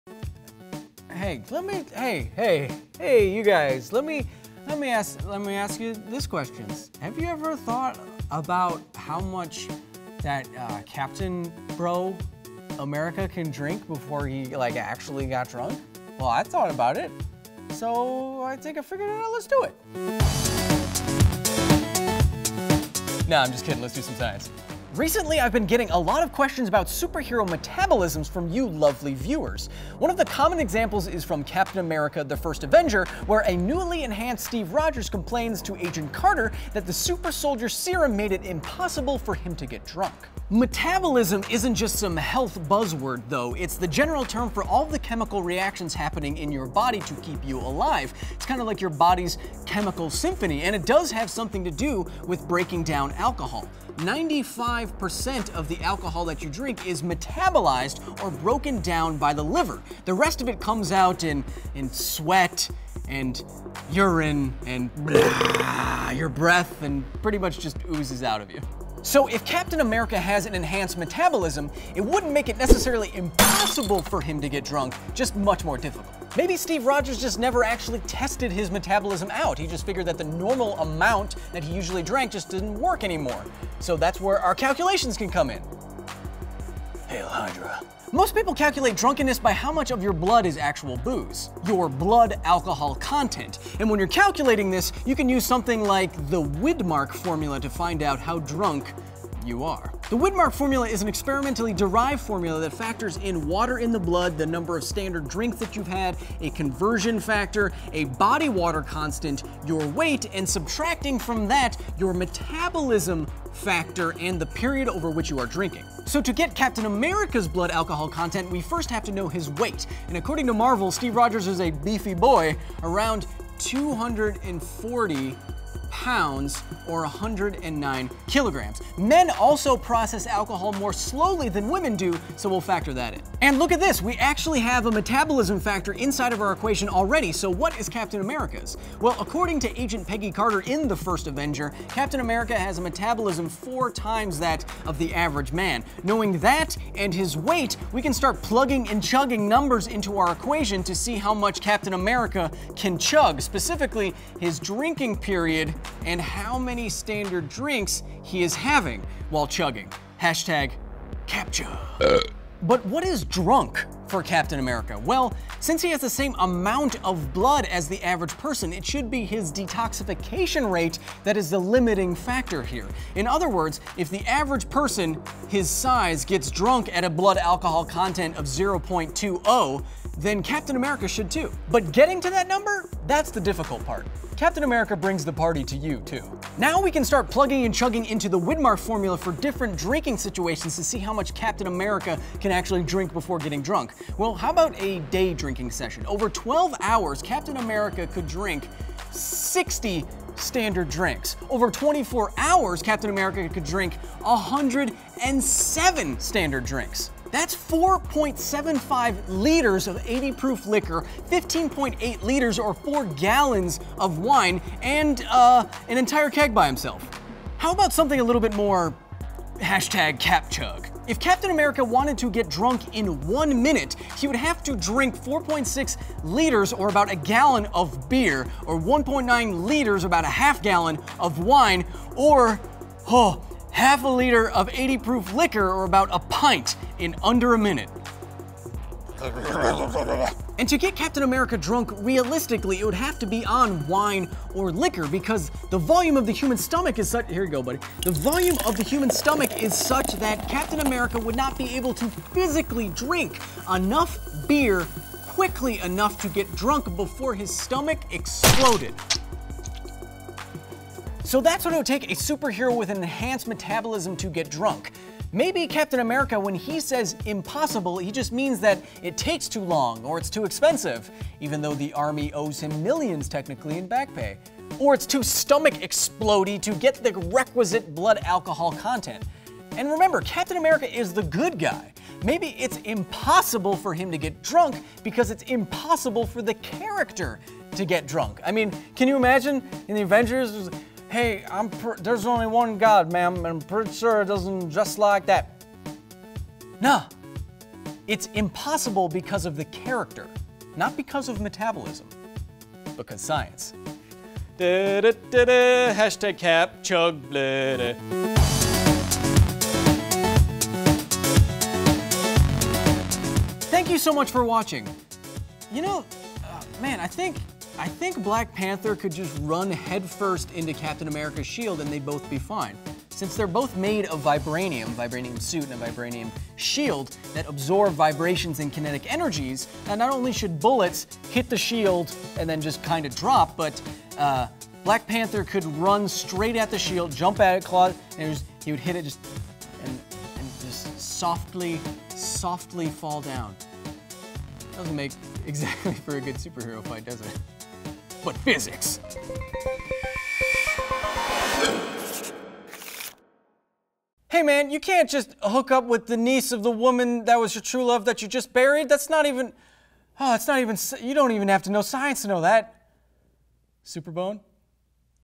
How Much Can Captain America Drink? (Because Science w/ Kyle Hill)
Watch Kyle Hill in The S.P.A.A.C.E. Program, only on Alpha! Free 30 day trial

Thanks to his increased metabolism from the Super Soldier serum, Captain America can’t get drunk… or can he? Kyle does the alcoholic math on this week’s Because Science!

Because Science every Thursday morning.